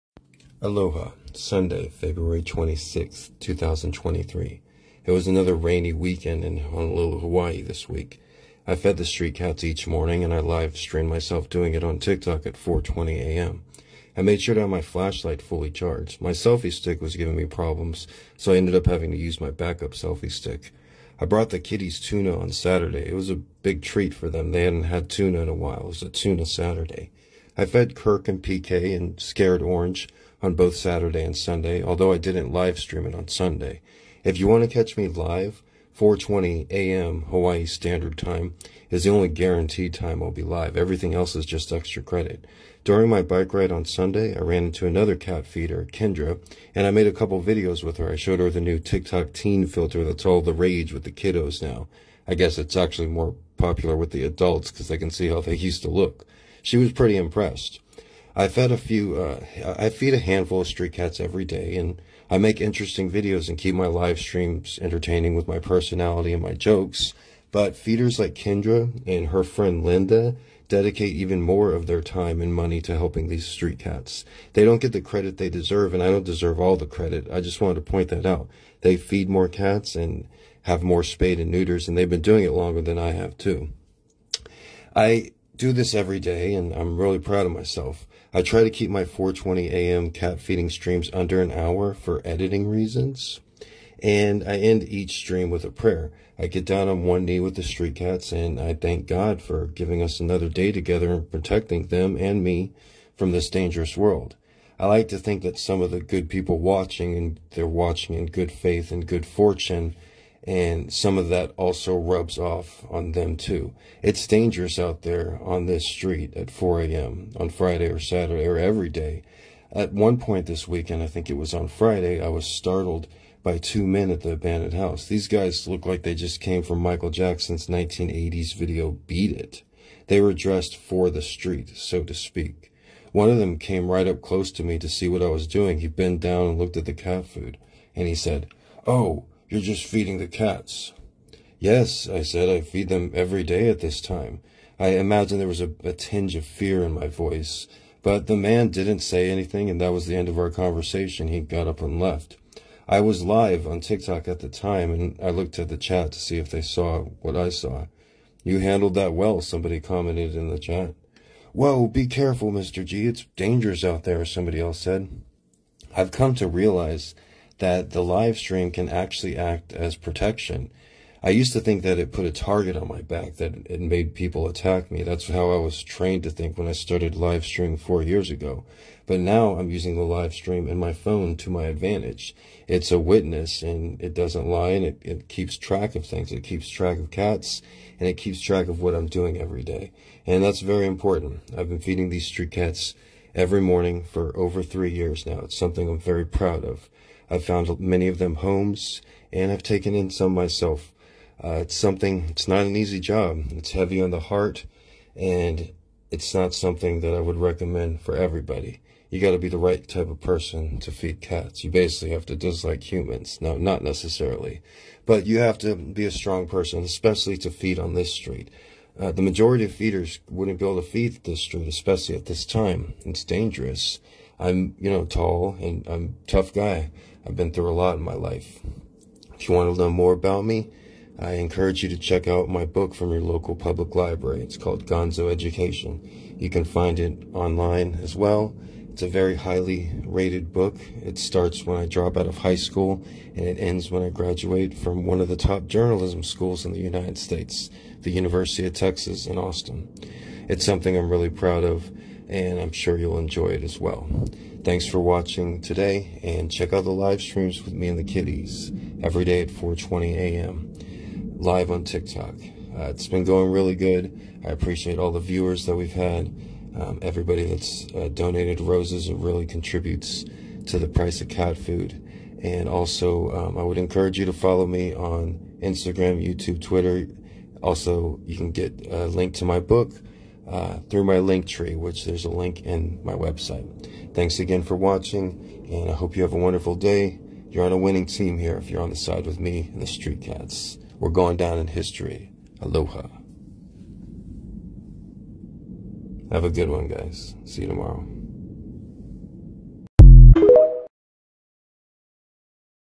Street Cat Feedin' || Honolulu, Hawaii || Sunday 4:20am February 26th 2023
Gregory is a writer in Honolulu, Hawaii. He feeds a colony of cats every morning before sunrise.